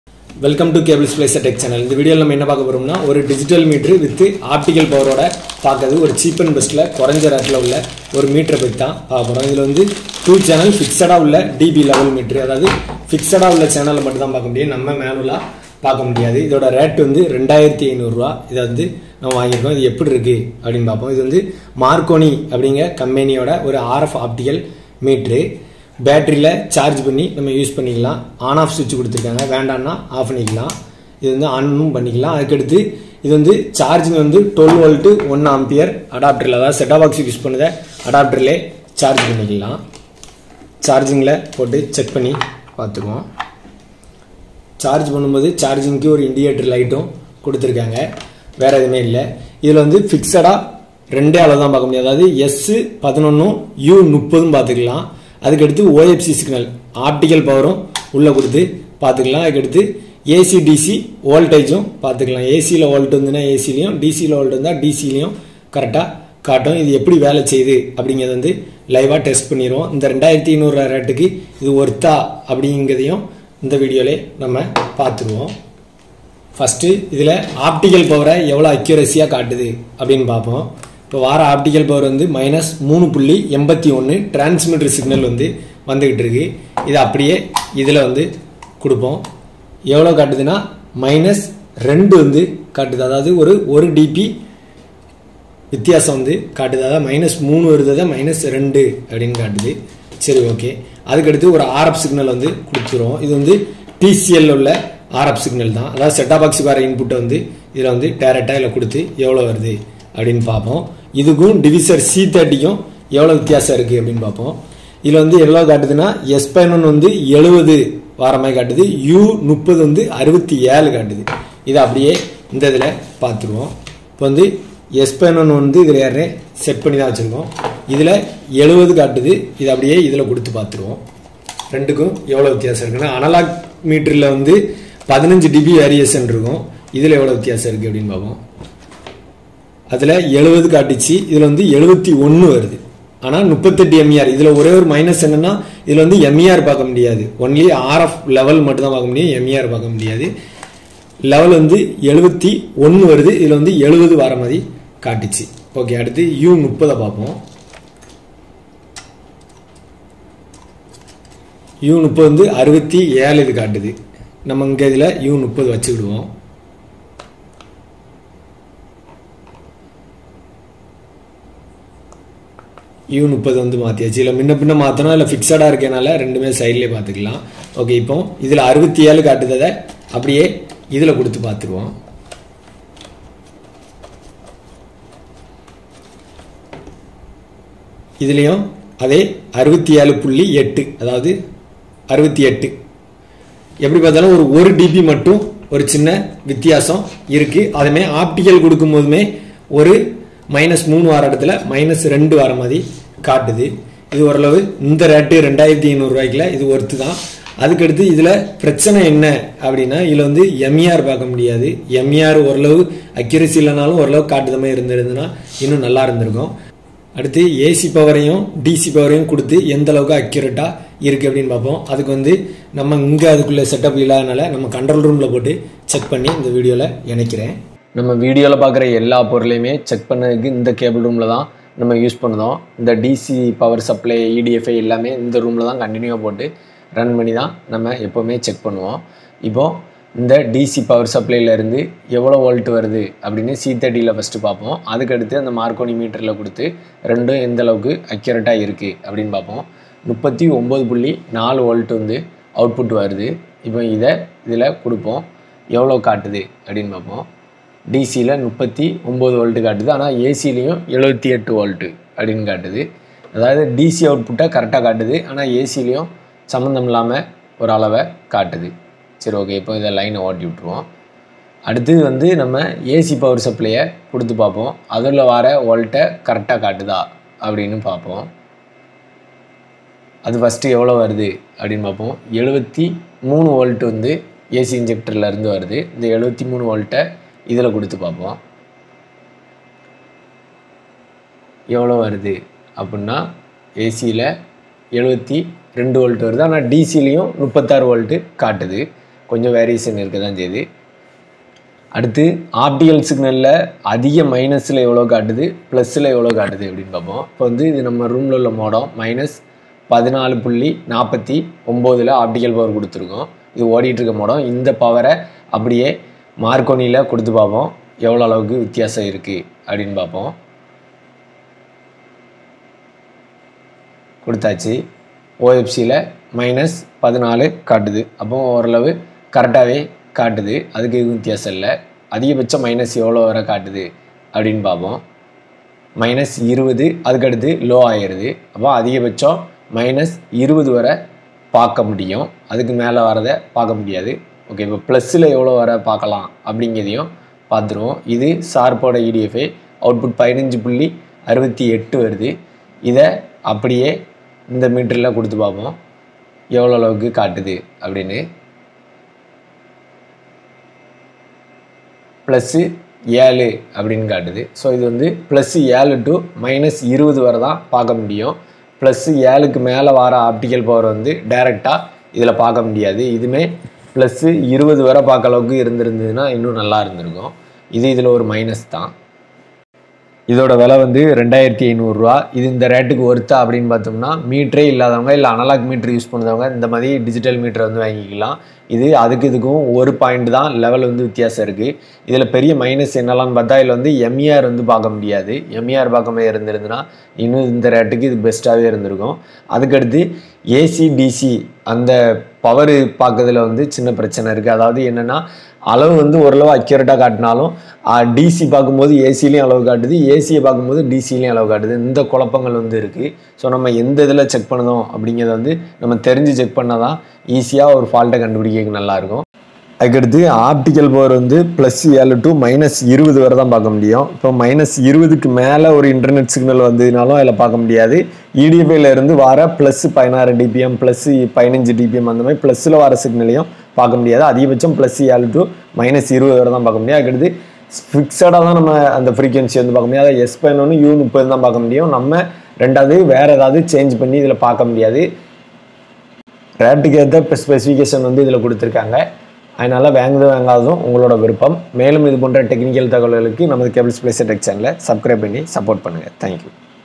Low Budget Optical + RF Digital Meter for Cable TV | Accuracy Live Testing | Marconi vs Deviser C30
☑️ What are the differences between DEVISER C30 Meter and Marconi RF + Optic Digital Meter ??? Which shows the exact size..

☑️ Compare the results of Marconi meter for Rs 2500 in this video

☑️ DEVISER C30 மீட்டர்க்கும் Marconi RF + Optic Digital மீட்டர்க்கும்  என்ன என்ன வித்தியாசங்கள் ??? எது அளவு துல்லியமாக காட்டுகிறது..

☑️ 2500 ரூபாய் விலைக்கு மார்க்கோனி மீட்டர் சிறந்ததா என்பதை அதன் முடிவுகளை ஒப்பிட்டு இந்த வீடியோவில் காணுங்கள்